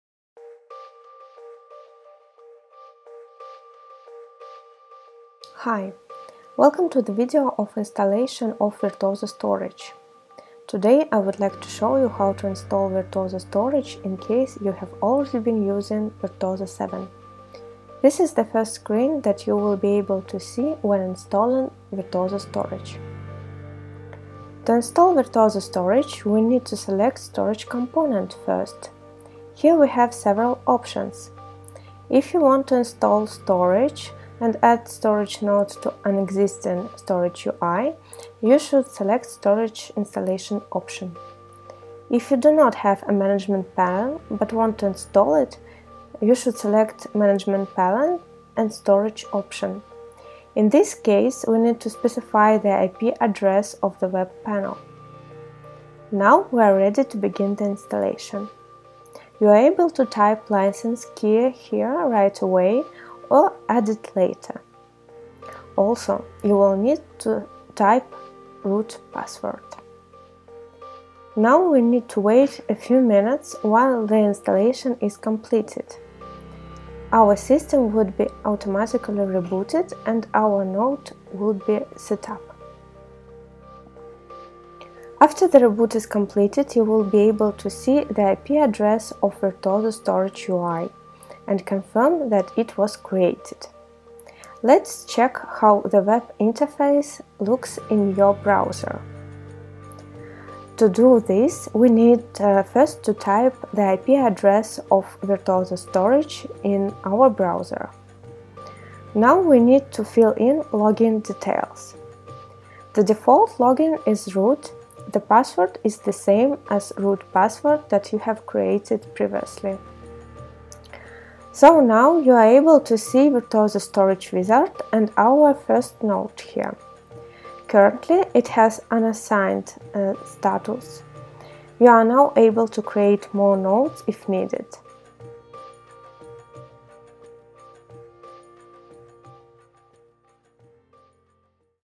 Virtuozzo Storage Installation Guide – Part 1
Watch the first video to learn how to install Virtuozzo Storage. In this video, Virtuozzo Product Marketing Manager, Anastasia Dobrodeeva, shows how to install management panel and shares the first steps to get started with Virtuozzo Storage.